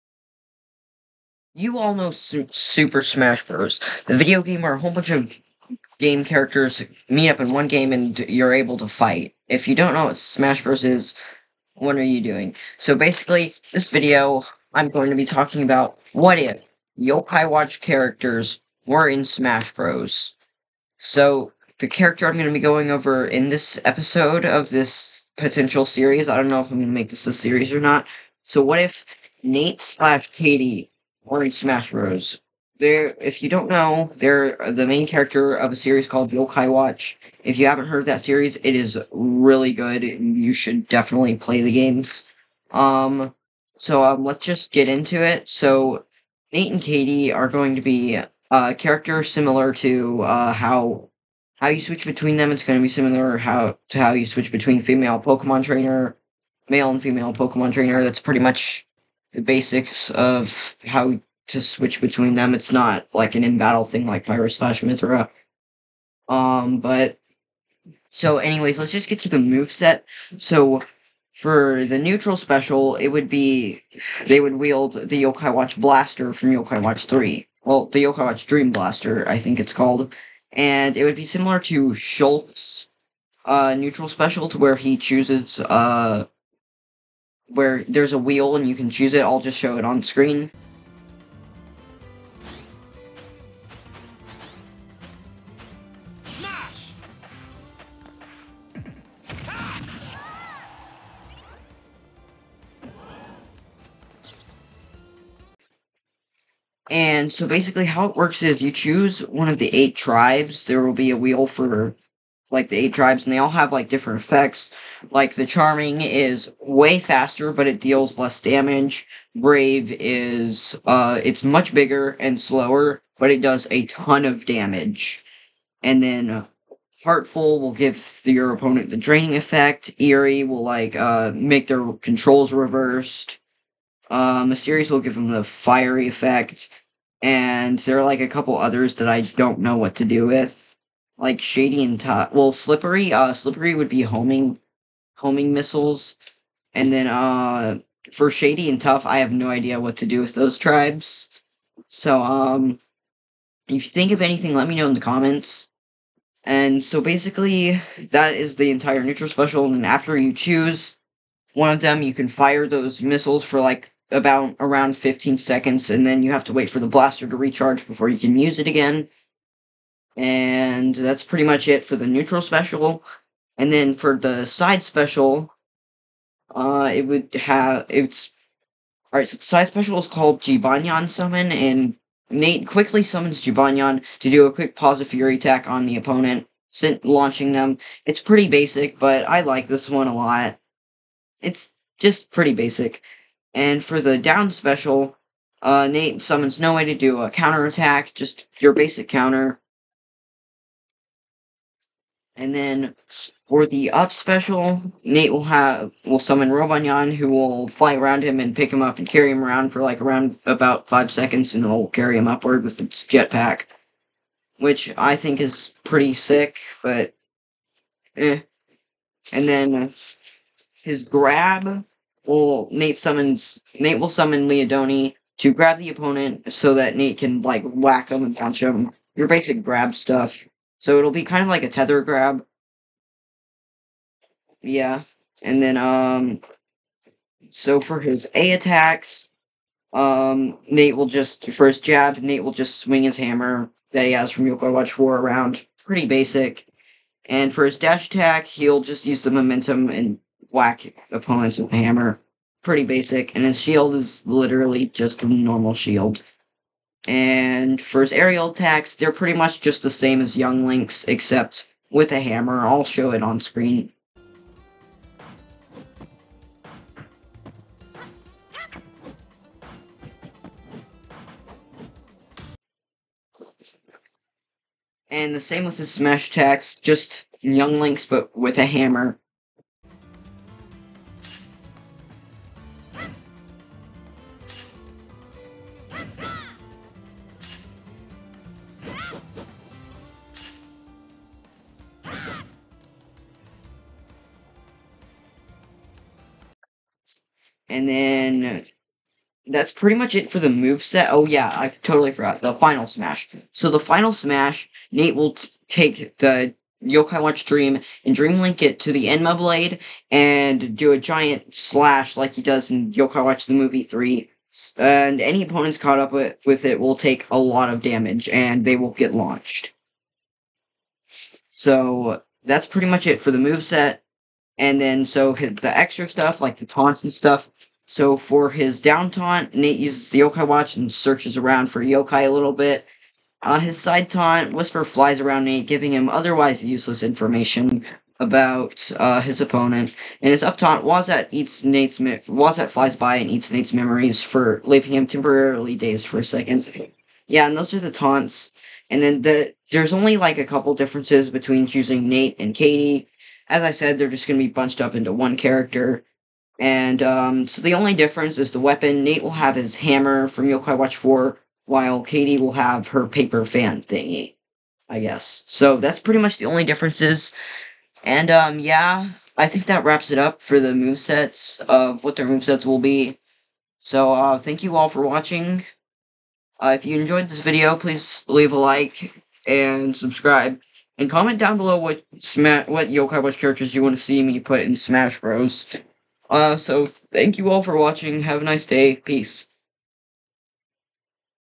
What if Yo-kai watch characters were in smash bros. Pt.1
Move set ideas for Nate/Katie if they were in smash bros.